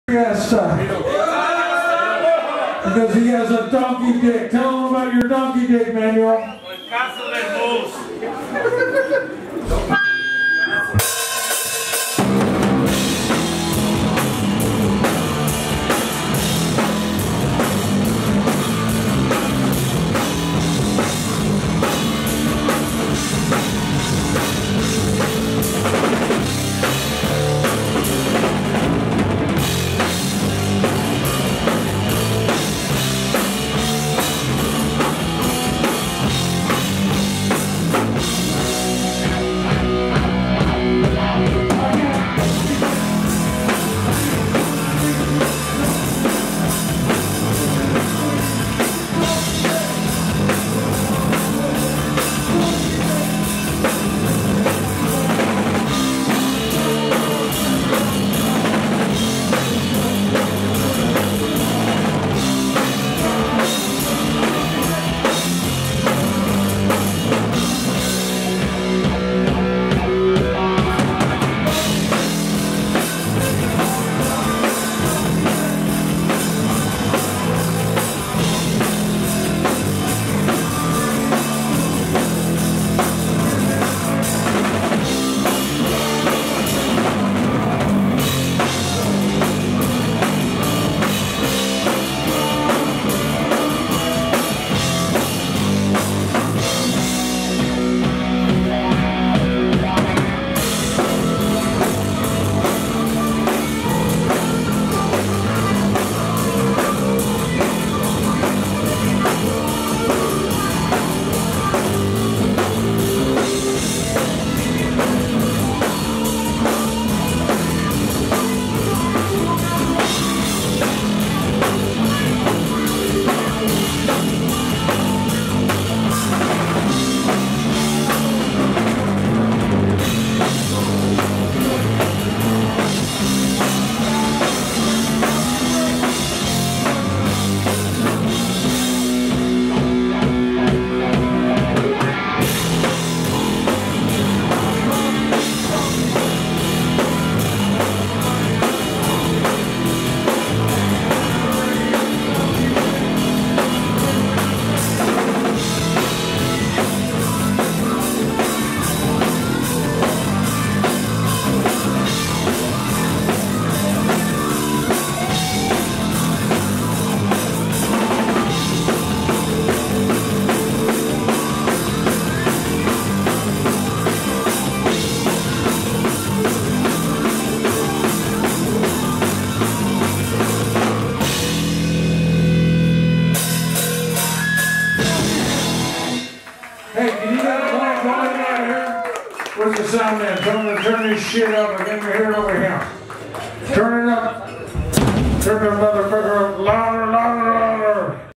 The Mentors live at Full Gass - video 1
13/04/2018
Steve Broy "Dr. Heathen Scum" - bass
Don Nutz - drums
Alessandro Tondakia - guitar